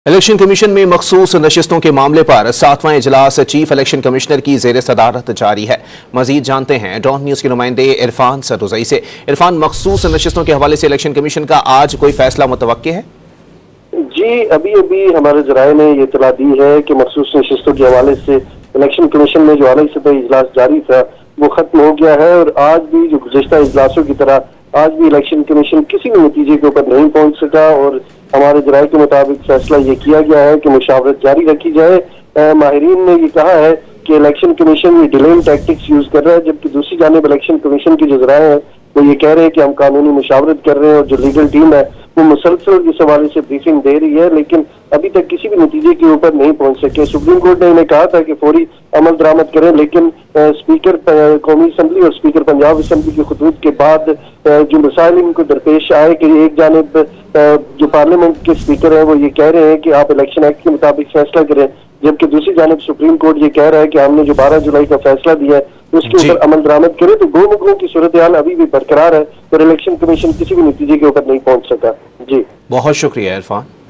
Big News From Election Commission On Reserve Seats | Breaking News | Dawn News
مخصوص نشستیں، الیکشن کمیشن سے بڑی خبر